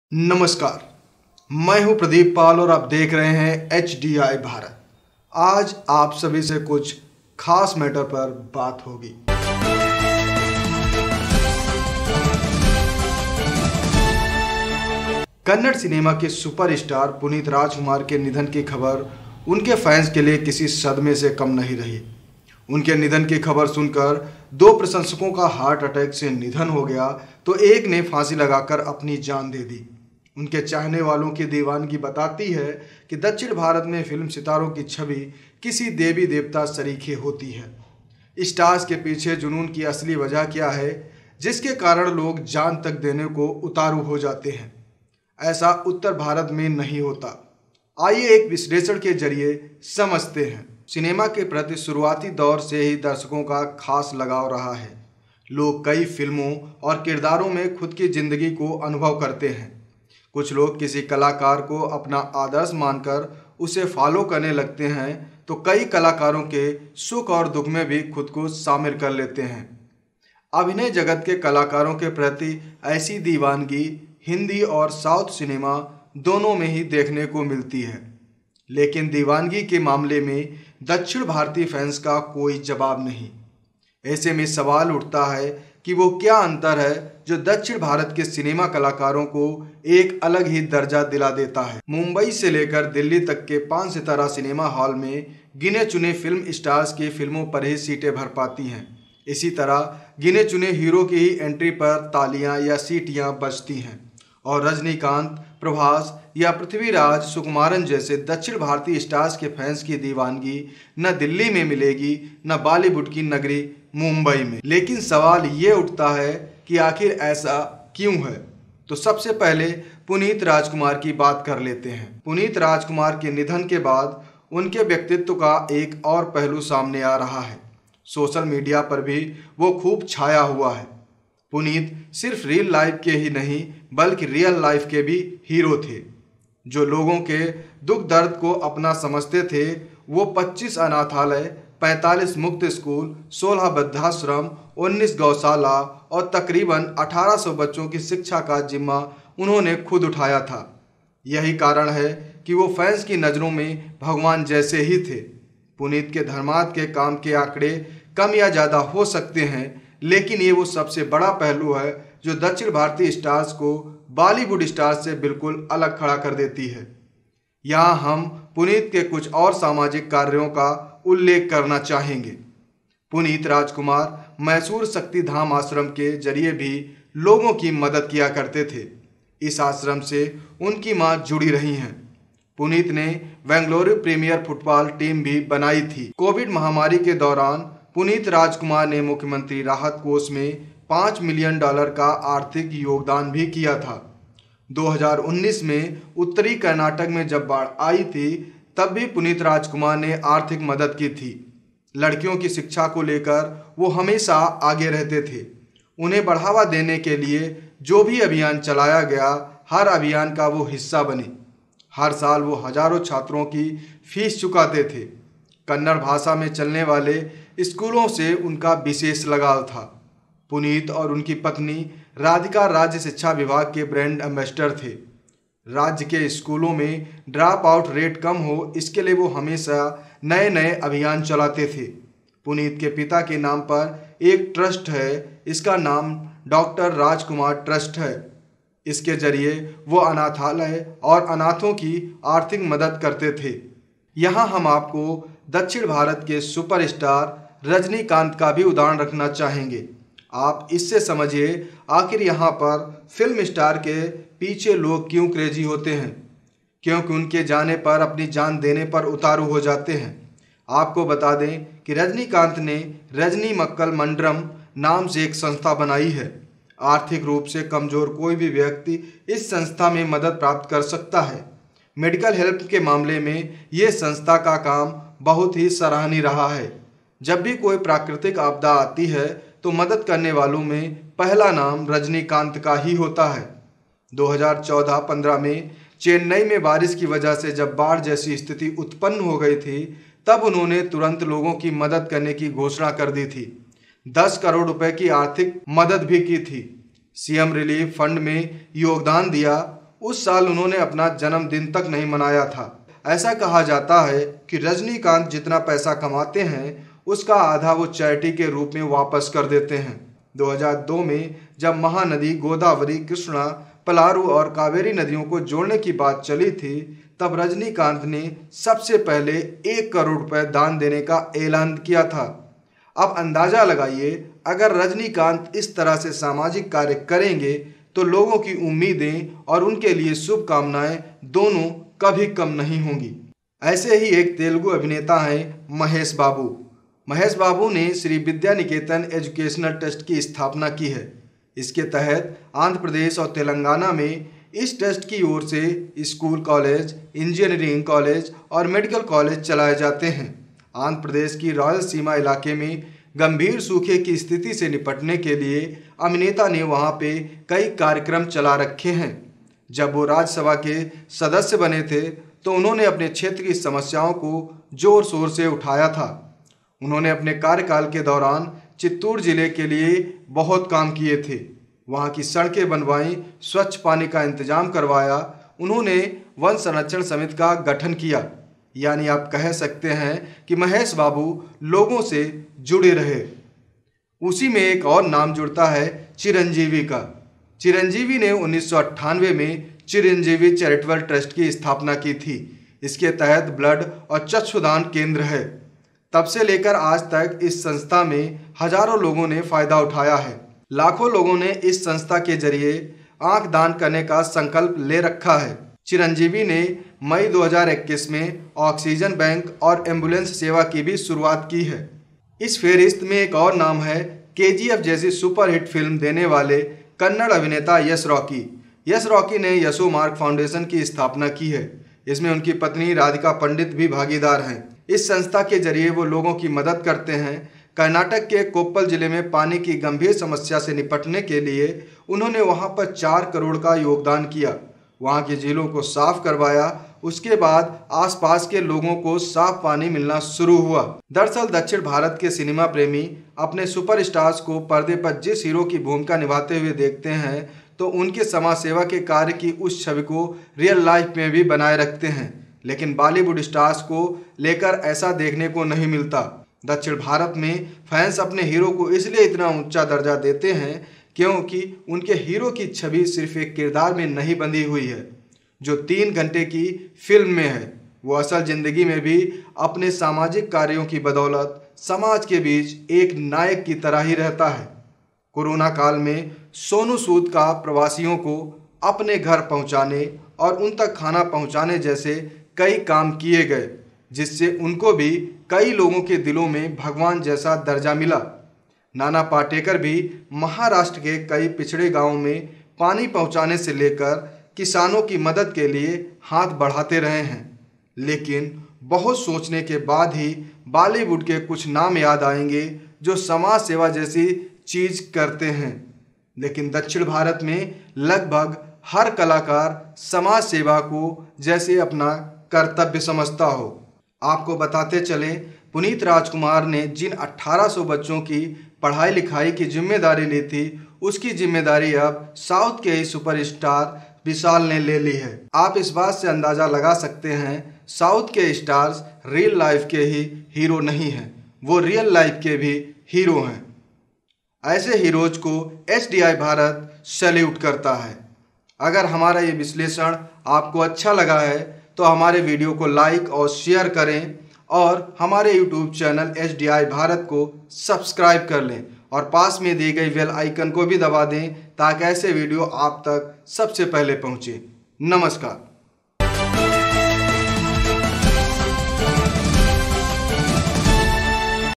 दक्षिण भारत के फिल्म स्टार्स को फैन्स क्यों देते है 'भगवान' का दर्जा ?I PunitRajkumar I HDI Bharat
कन्नड़ सिनेमा के सुपरस्टार पुनीत राजकुमार के निधन की खबर उनके फैंस के लिए किसी सदमा से कम नहीं रहा. उनके निधन की खबर सुनकर दो प्रशंसकों का हार्ट अटैक से निधन हो गया. एक ने फांसी लगाकर अपनी जान दे दी. उनके चाहने वालों की दीवानगी बताती है कि दक्षिण भारत में फिल्म सितारों की छवि किसी देवी-देवता सरीखे होती है. स्टार के पीछे जुनून की असली वजह क्या है, जिसके कारण लोग जान तक देने को उतारू हो जाते हैं. ऐसा उत्तर भारत में नहीं होता है.